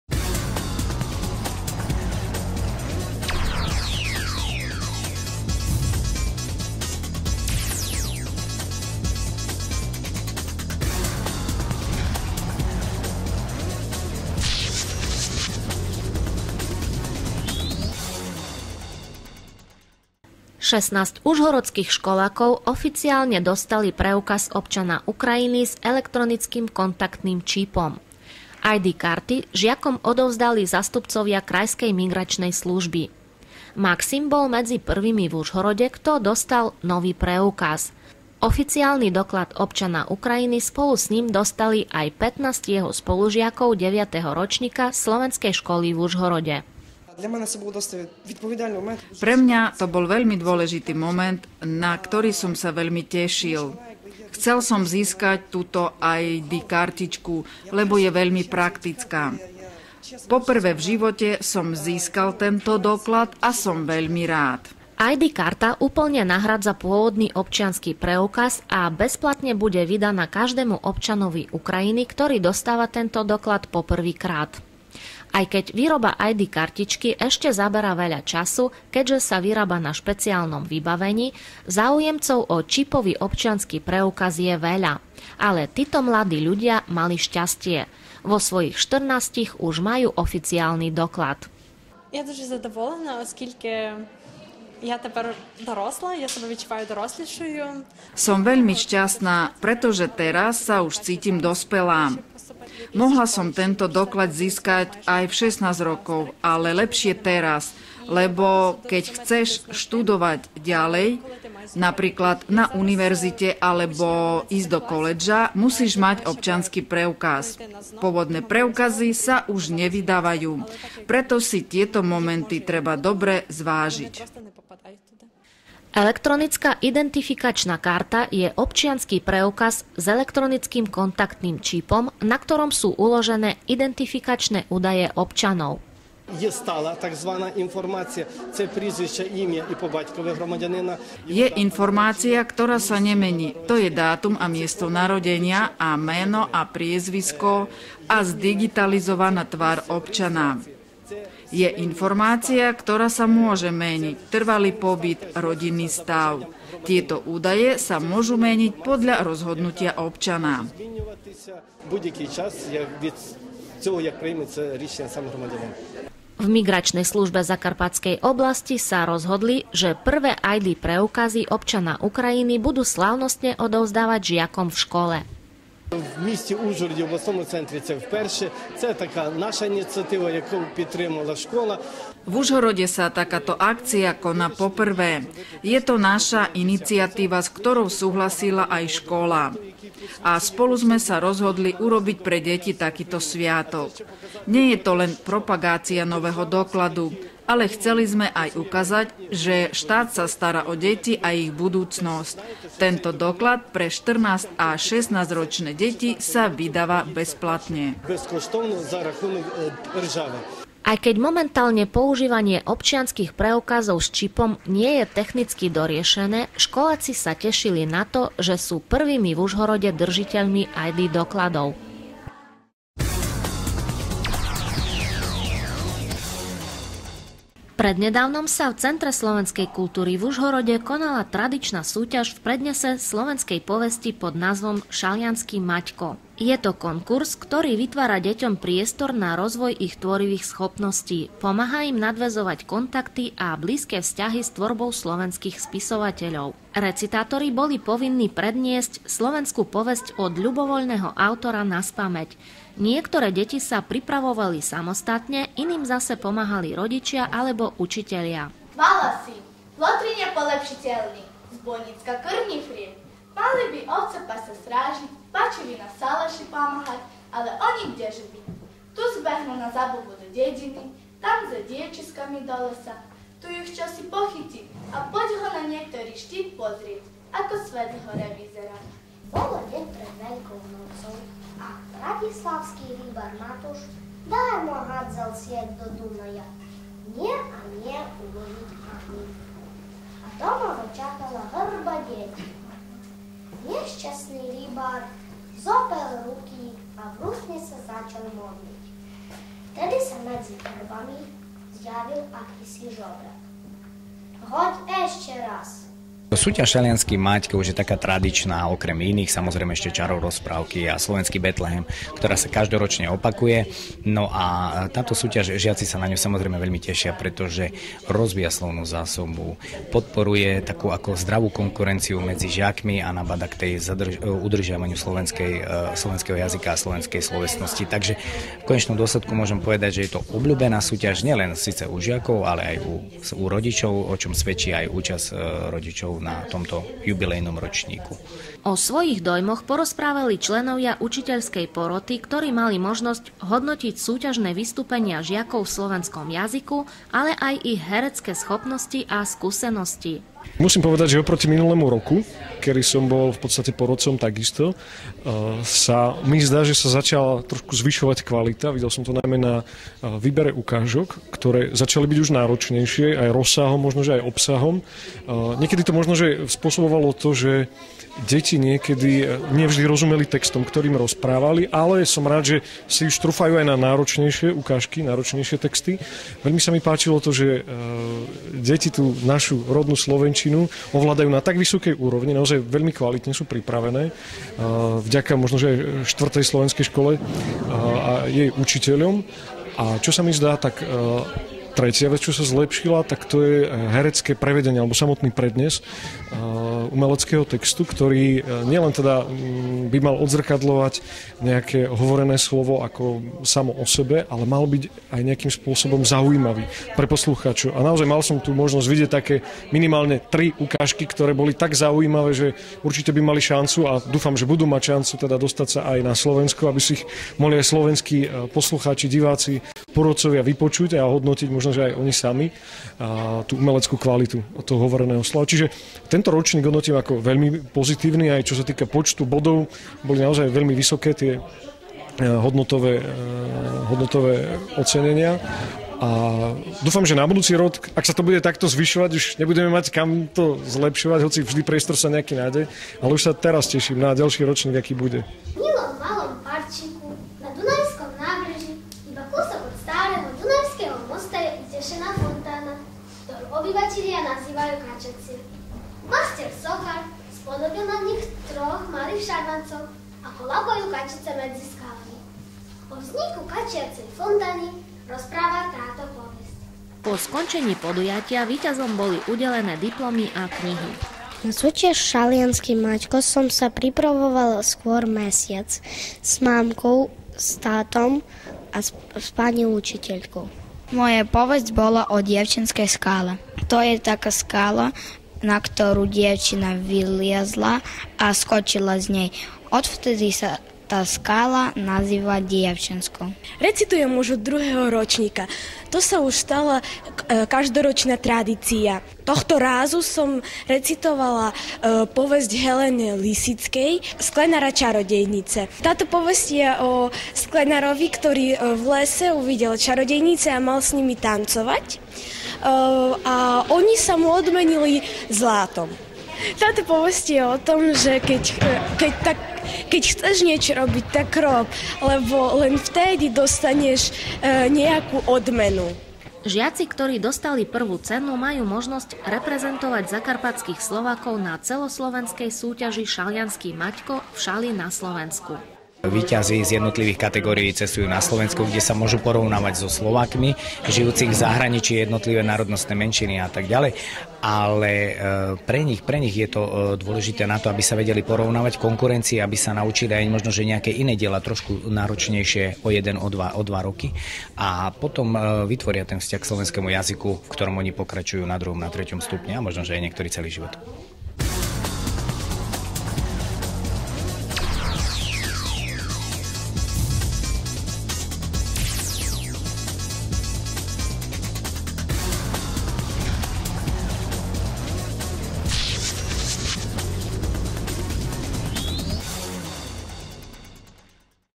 «Актуально». Програма словацькою мовою (17.02.17)
Дивіться в програмі:
Вручення паспортів громадянина України нового зразка учням словацької школи в Ужгороді
Конкурс декламування словацької повісті.